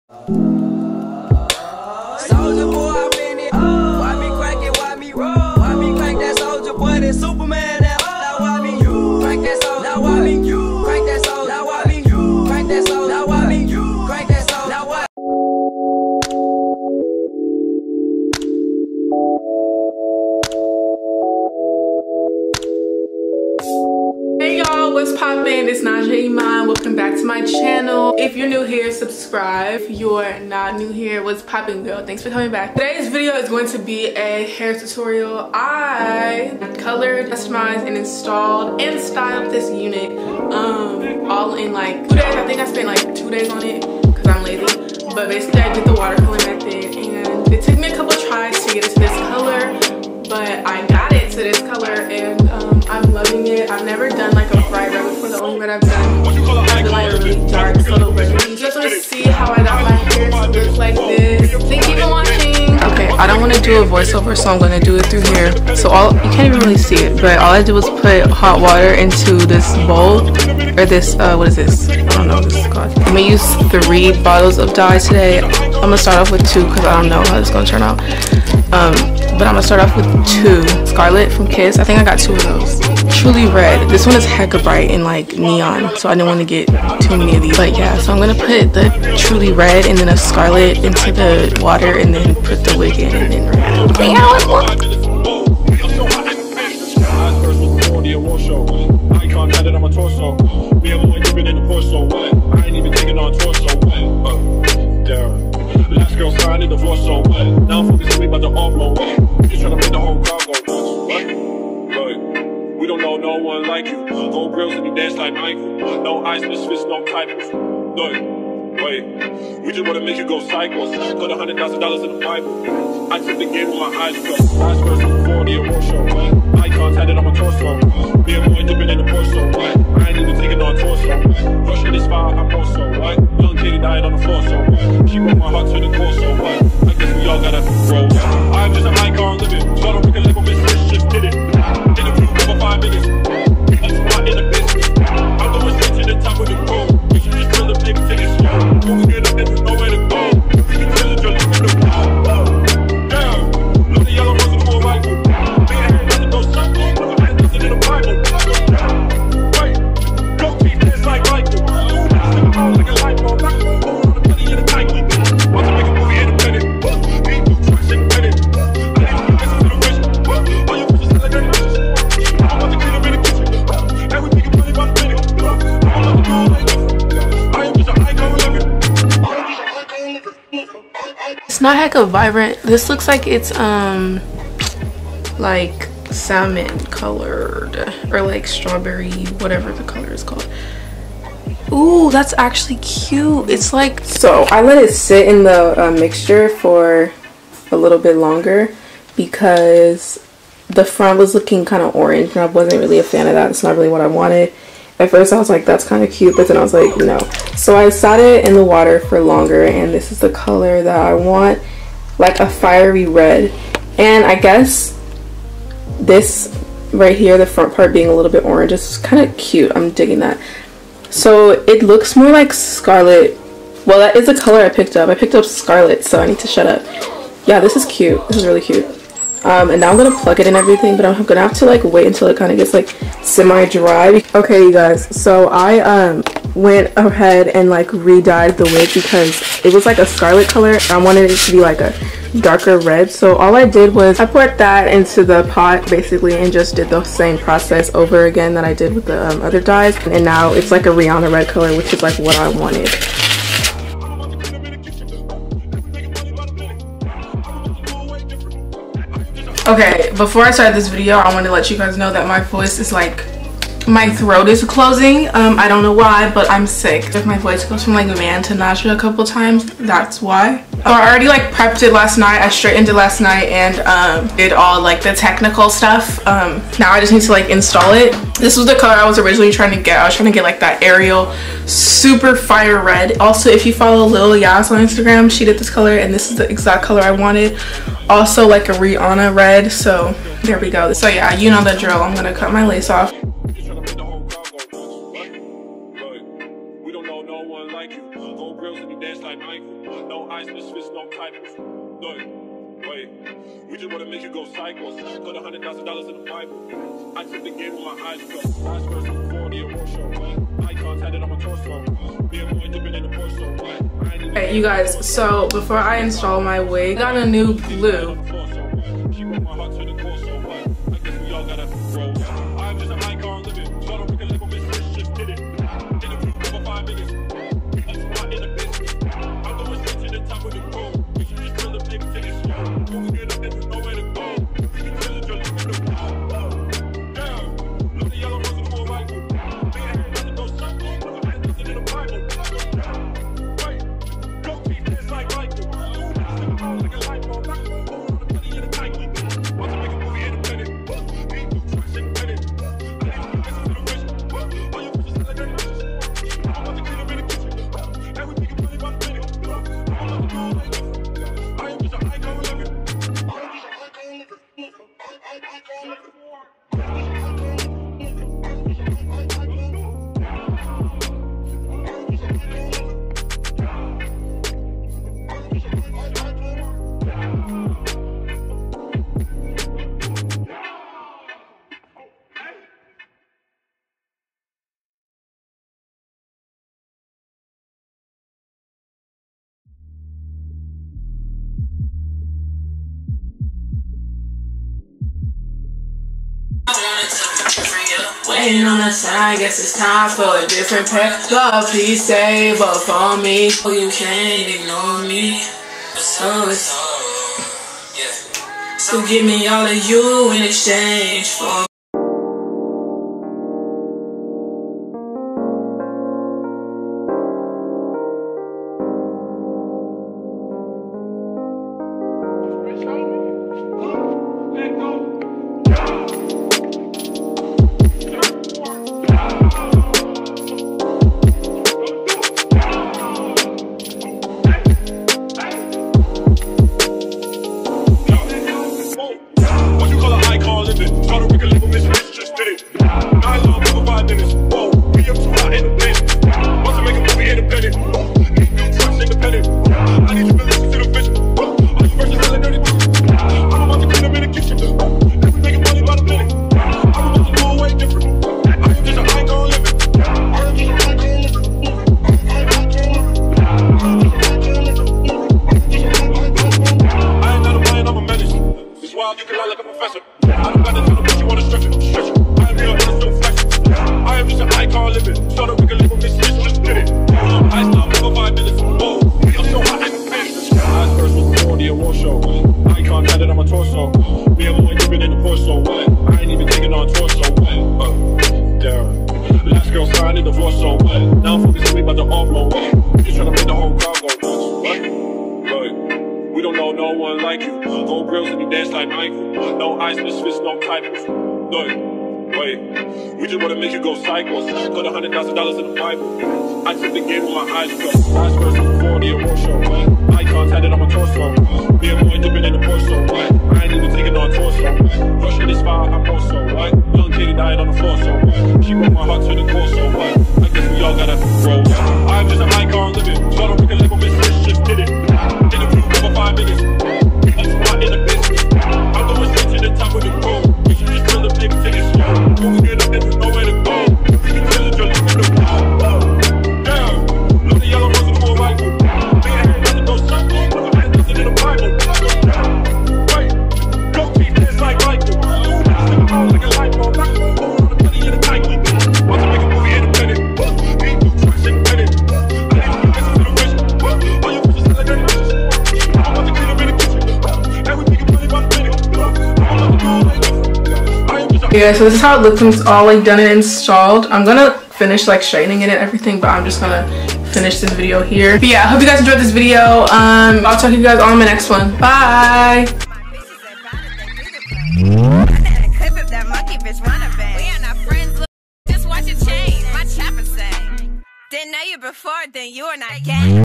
FIRE RED WIG START TO FINISH CUSTOMIZATION & INSTALL
dyes used: - 2 kiss express scarlet
                    - kiss express truly red
                    - 2 adore wild cherry

Video Wig Information:
Hair color: 613#
Hair Length: 20
Hair Density: 180%

Doubleleafwig Sales:
Over 189$ get 20$ off, code: Love20
Over 289$ get 35$ off, code: Love35